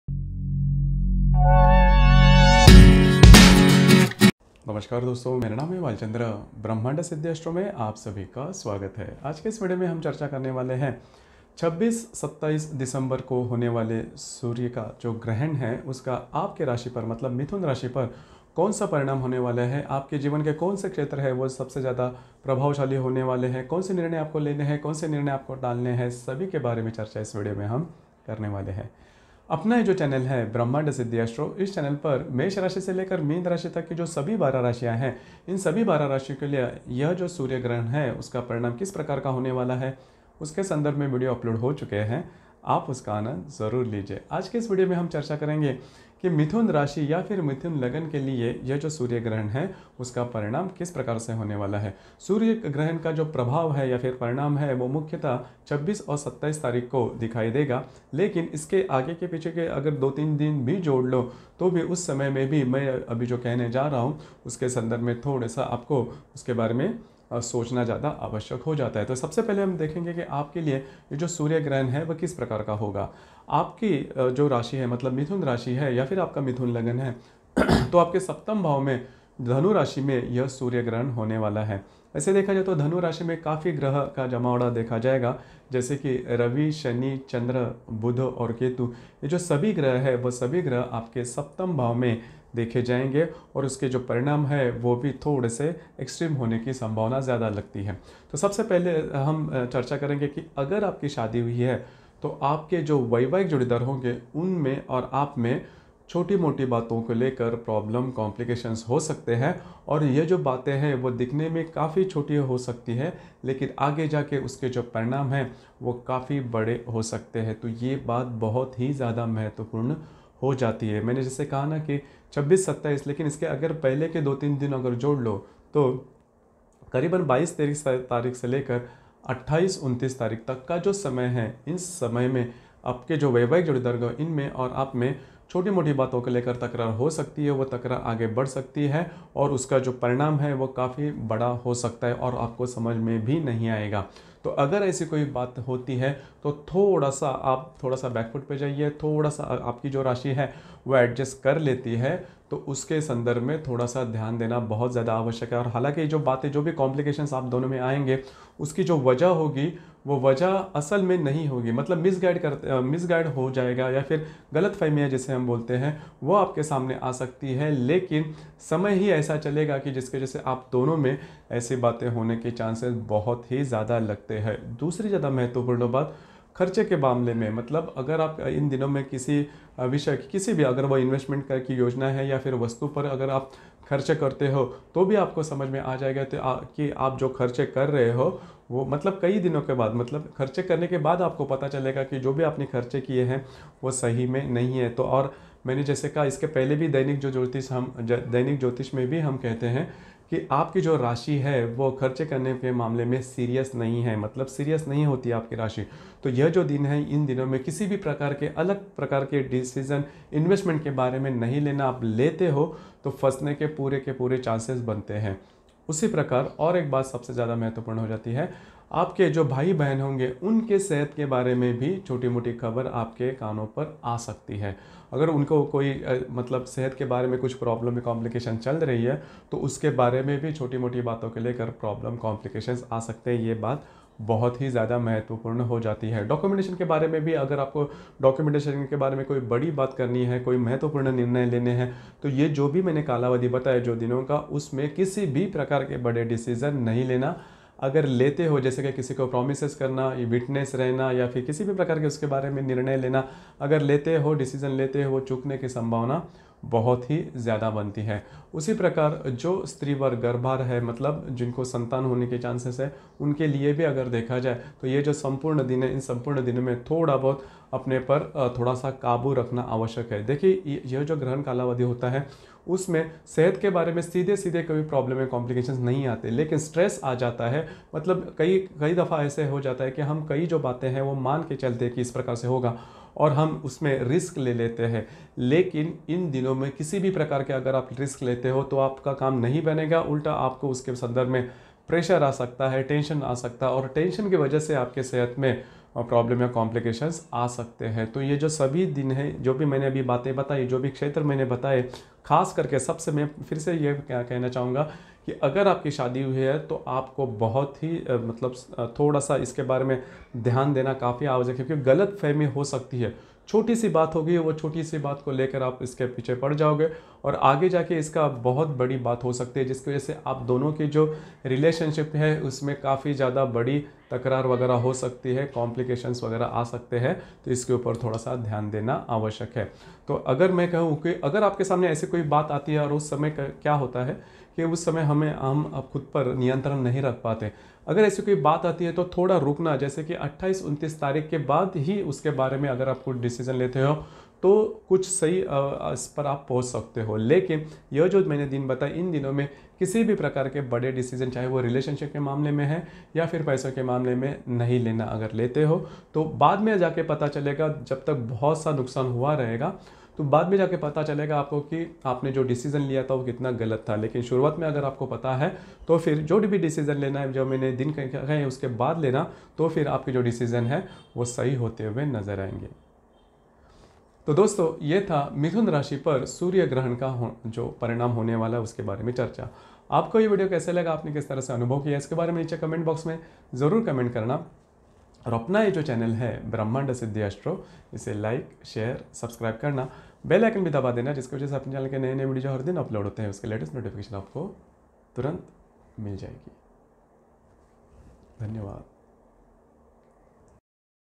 सूर्य ग्रहण का असर मिथुन राशिपर
इस वीडियो में सूर्य ग्रहण का असर मिथुन राशि की जीवन को किस प्रकार प्रभावित करने वाला है यह चर्चा की गयी है